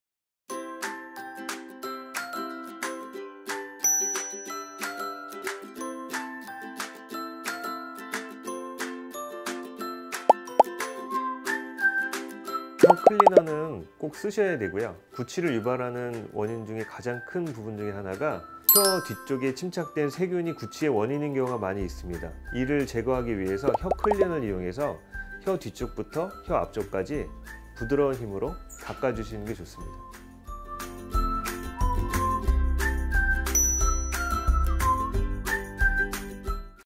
구강관리 혀 클리너가 도움이 될까? (Can oral care tongue cleaners help?)
최근에는 많은 구강관리용품이 나오고 있어
구강관리를 위해 다양한 것을 사용하는데요.

그중 혀 클리너가 과연 치아 건강 관리에
도움이 되는 것인지 궁금해 하는 분들이 많더라고요.

그래서 연세닥터연치과 연제영원장님은
과연 혀 클리너가 도움이 되는지에 대해
영상을 통해 설명해 주셨어요.

함께 영상을 확인해 볼까요?

There are a lot of oral care products these days.
That's why many people use a variety of things for oral care.
Among them, many people wonder if tongue cleaner can help with
dental health care.
So, the dentist explained through a video about how tongue cleaner
can help with tooth care. Shall we check the video together?

오시는 길▶ 서울시 강남구 논현로872 영빌딩2층
                       압구정역(3호선) 2번출구